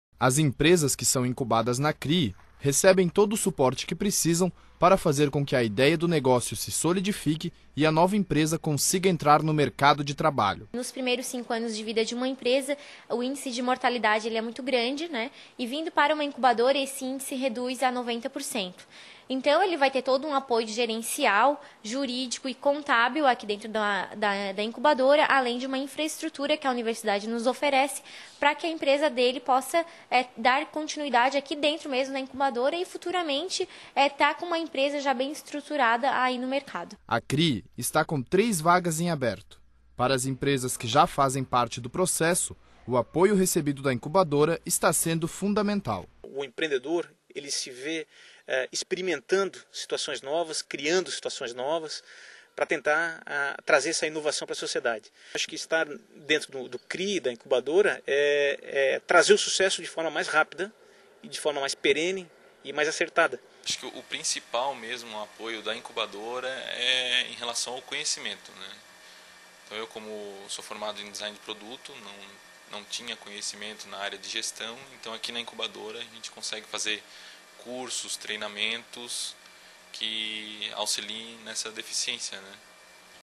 Crie tem vagas abertas para novos projetos
Se você tem uma ideia original para uma empresa ou produto e não sabe por onde começar, o Centro Regional de Inovação e Empreendedorismo da Unisul, Crie, pode ajudá-lo nesse processo. A incubadora de empresas da universidade está com vagas abertas